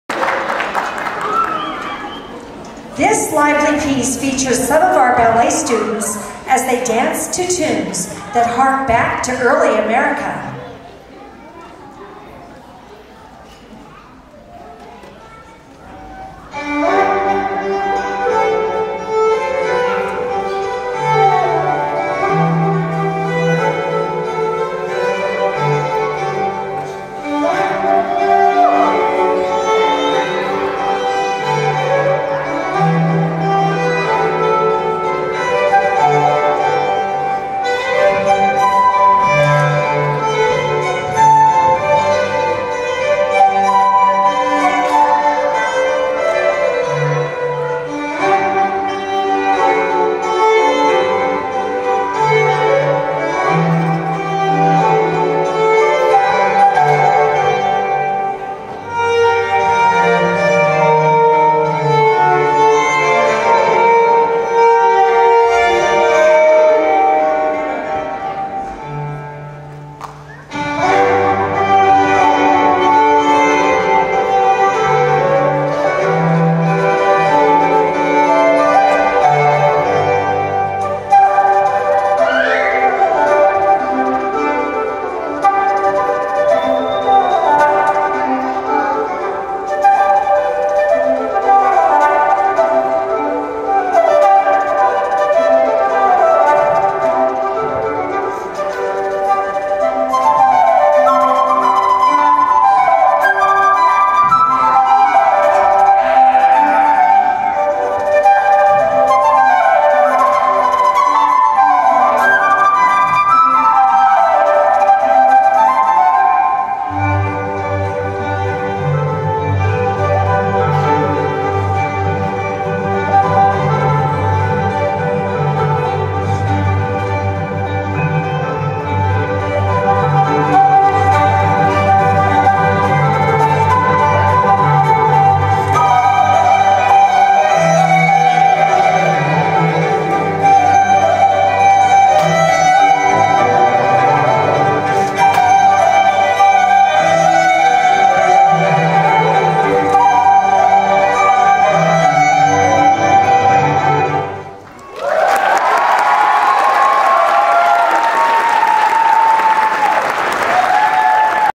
Shellie 2nd FR Dance Recital Jun2022 0010
Shellie 2nd of 2 performances today at the FR Dance Recital Jun2022.

There was a 3:00 pm show and 6:30.  This is from the 6:30 at the Bob Boldrick Theatre. Carson City Community Theater.  She had a great time!